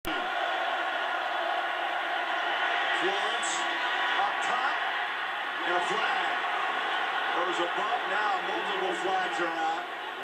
More flags, please
Many officials agree Karl Joseph committed pass interference on this play. FOotage property of FX.